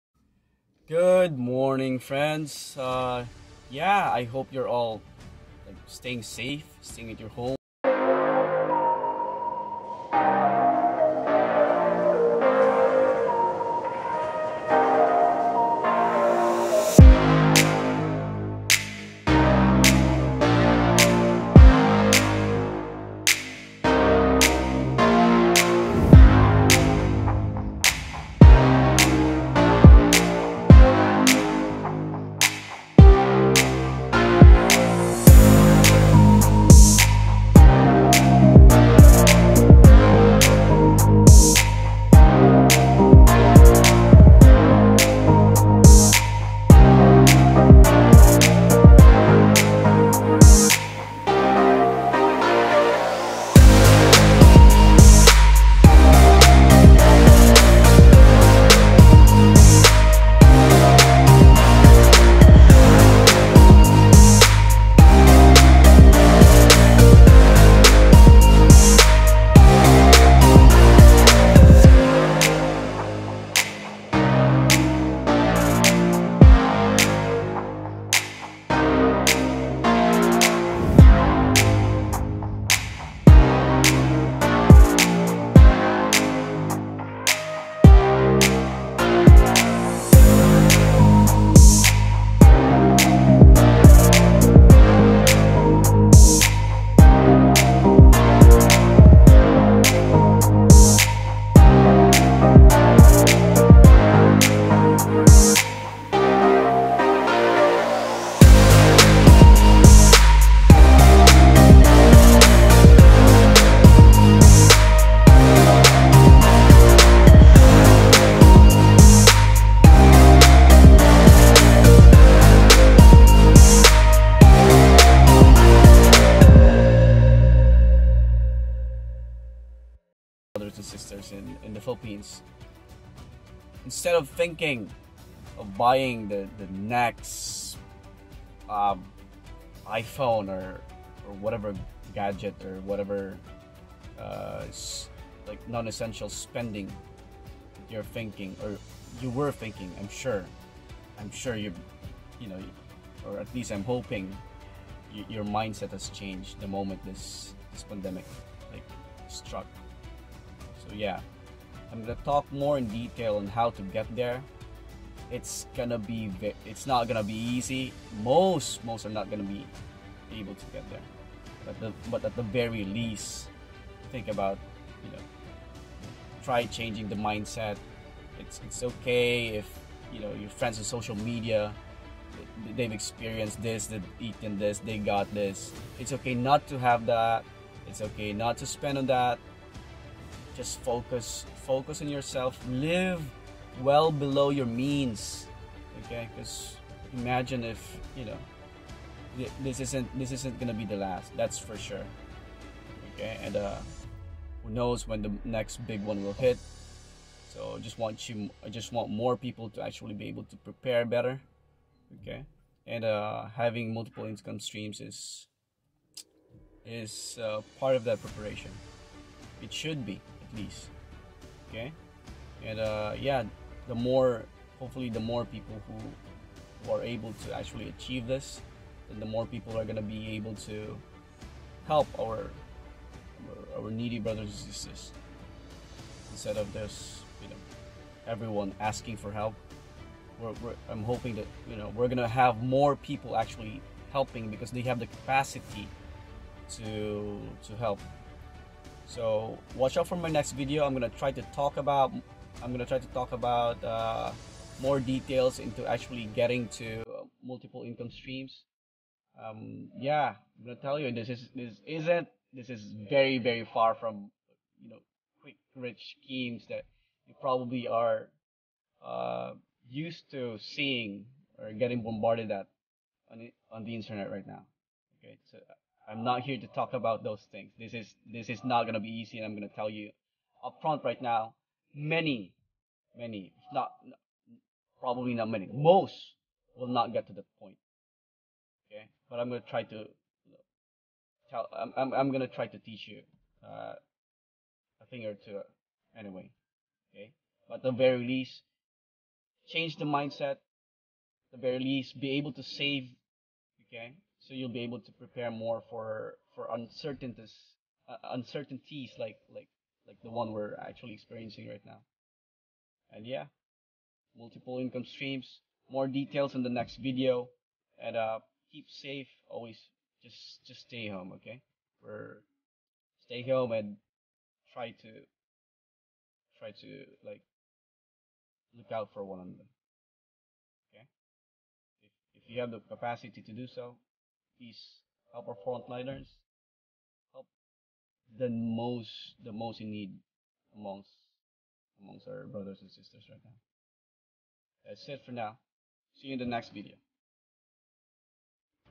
Multiple income streams - a need covid19 has highlighted.
I know things are looking bleak for so many of us right now, but I am hoping we can all still try to learn from this.

For my first episode in this covid-19 quarantine talk series, I hope many of us have come to realize the risks associated with having only one (1) income stream and will work harder to get this sorted out the soonest.

Please do like/share and comment if you have any questions whatsoever.